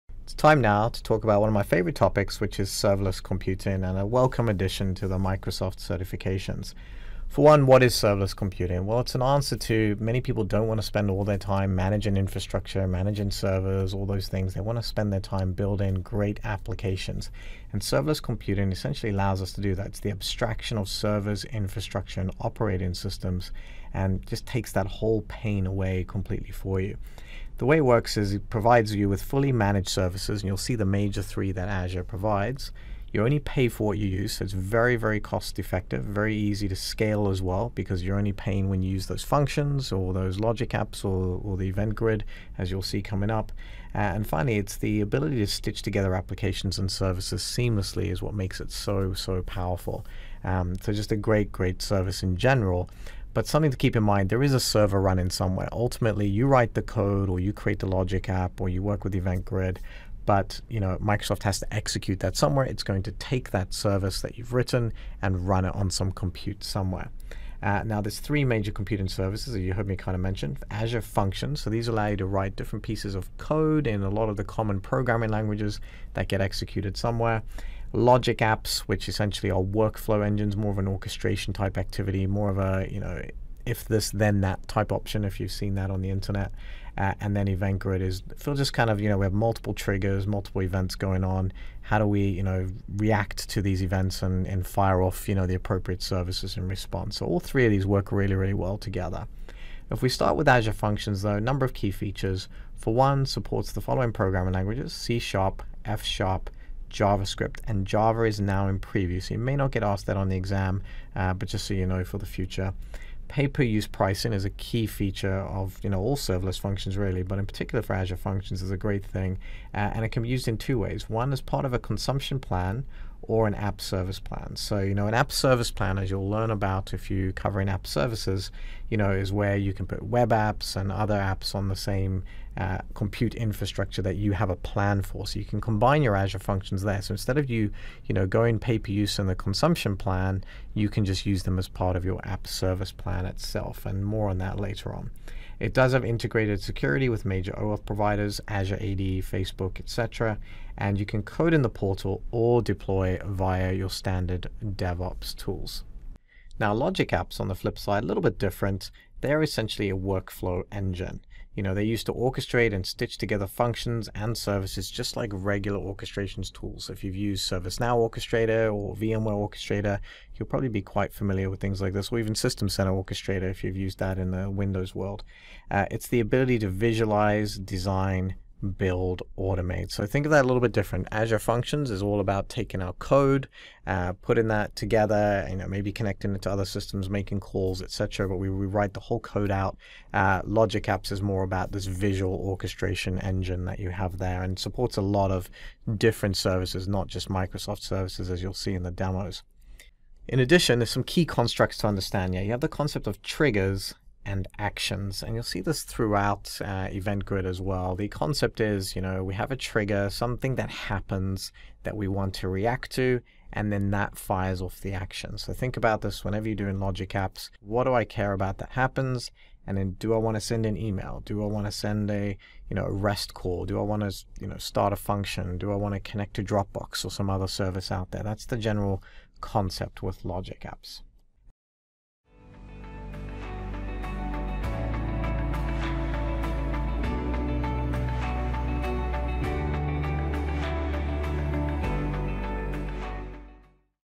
AZ-900 Azure Fundamentals: Serverless Computing Overview
This is a FREE lesson from our Skylines Academy AZ-900: Microsoft Azure Fundamentals course. In this lecture, you will learn about Serverless Computing, which provides you with fully managed services, allows you to pay for only what you use, and gives you the ability to stitch together applications seamlessly.

If you would like to take the complete course, head over